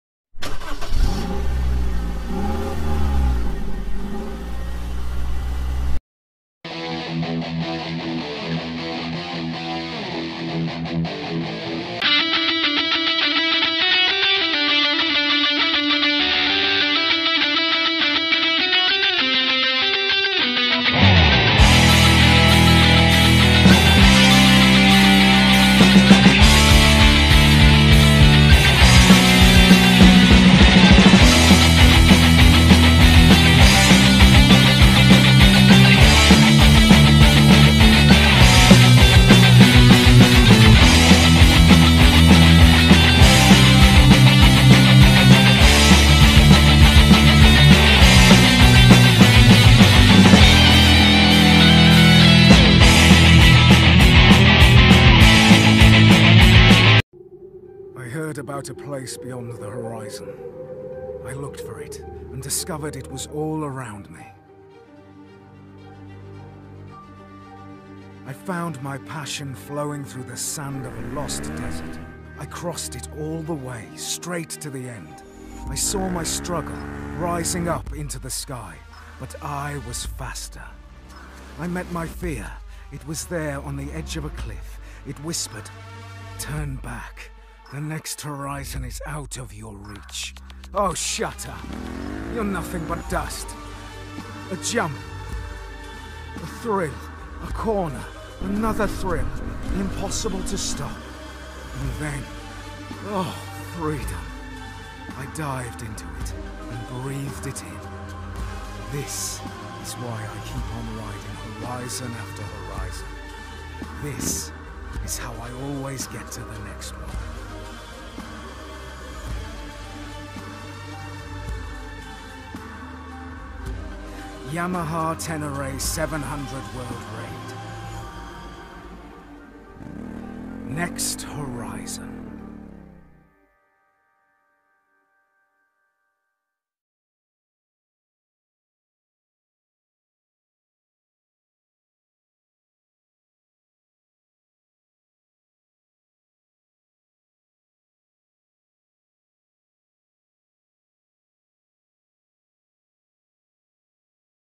THE NEW 2018 YAMAHA TENERE 700(with video)|K AUTO TECH
Hands down, one of the most exciting bikes at last year’s EICMA show in Milan, Italy, was the Yamaha T7 concept. We all love ADV bikes, but most of them are huge, heavy, and expensive, so the idea of a lightweight, long-travel rally rig built on the bones of the FZ-07 had our hearts racing.
That was 12 months ago, and in the last year interest in the T7 has never waned, so we fully expected Yamaha to announce a production model for 2018. No such luck. Instead, another concept, called the Ténéré 700 World Raid. A bunch of Yamaha test riders will get to ride this thing around the world next year as part of a “World Raid Tour," but there’s no production bike in sight. Come on, Yamaha!
The bike currently on display in Milan has the same profile of last year’s T7, but Yamaha has added more fuel to the fire of our fantasies by telling us the 689cc “CP2” parallel-twin engine was specially developed for this application, presumably with more midrange thrust.
K AUTO TECH